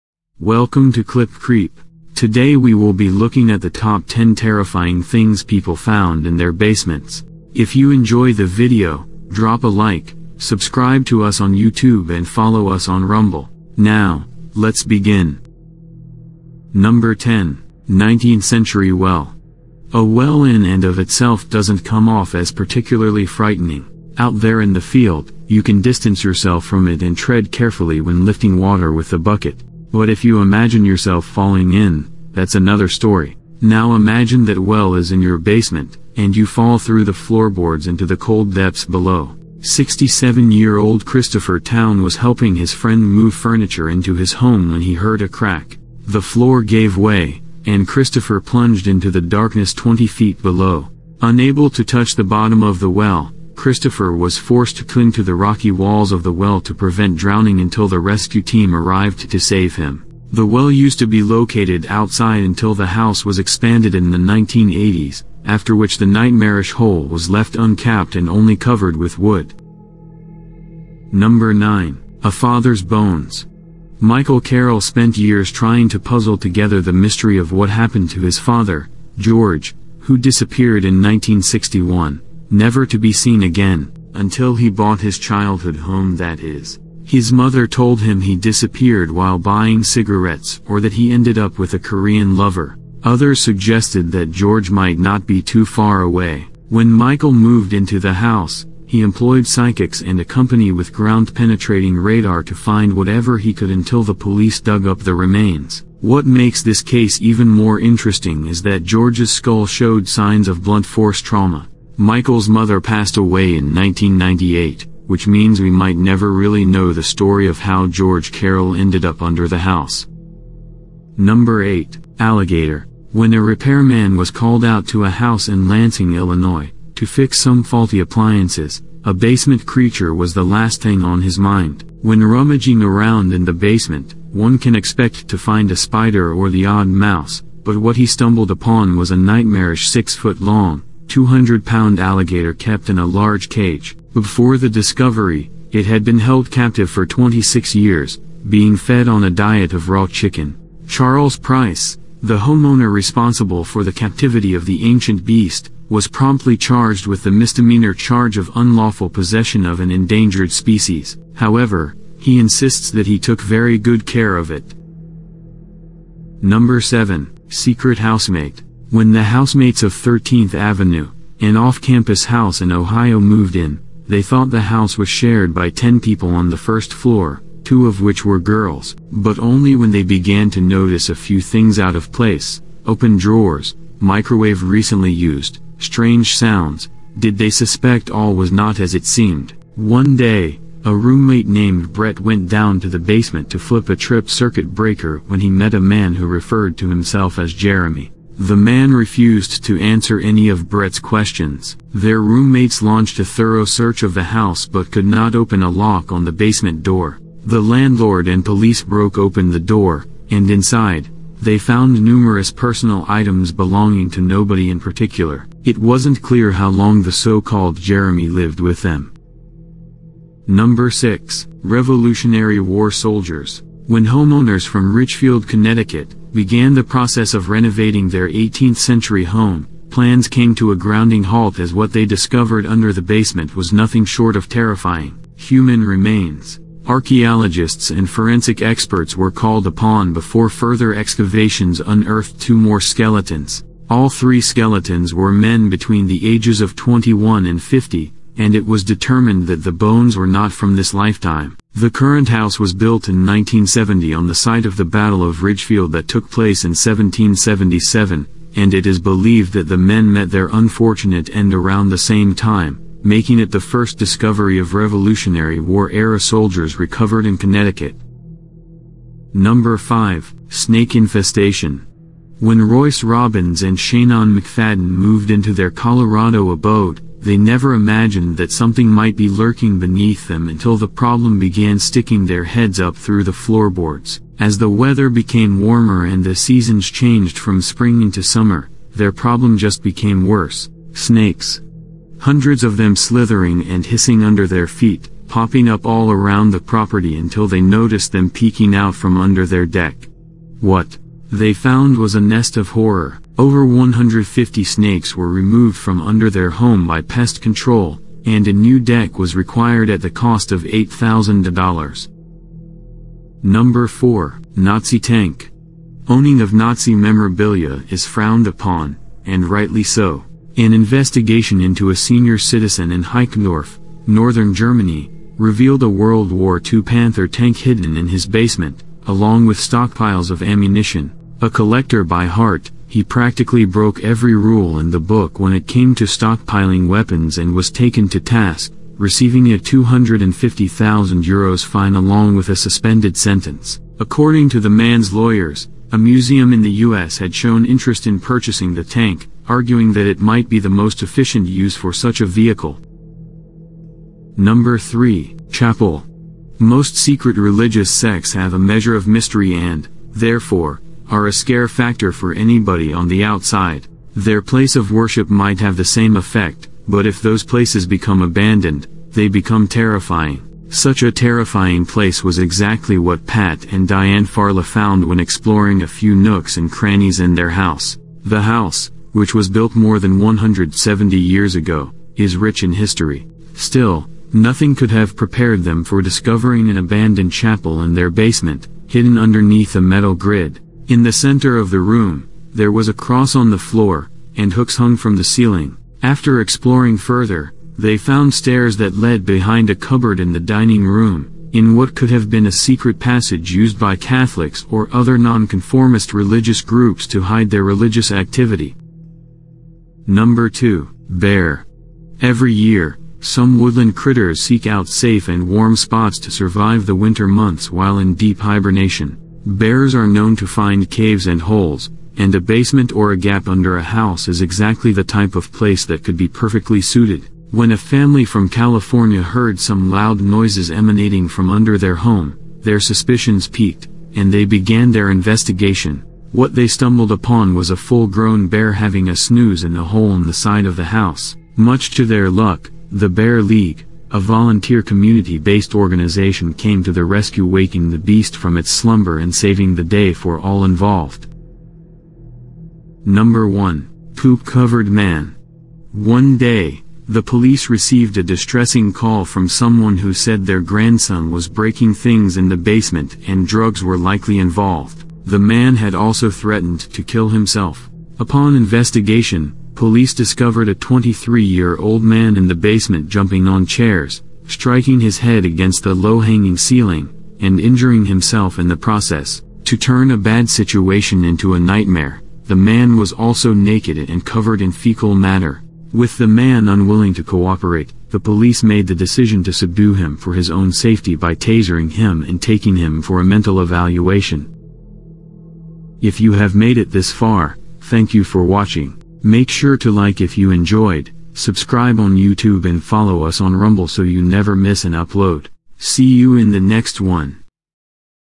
Top 10 Terrifying Things People Found In Their Basements
Basements:
0:00 Intro
0:16 Nineteenth-Century Well
1:10 A Father’s Bones
2:01 Alligator
2:45 Secret Housemate
3:39 Revolutionary War Soldiers
4:29 Snake Infestation
5:13 Nazi Tank
5:53 Chapel
6:49 Bear
7:32 Poop-Covered Man
8:16 Outro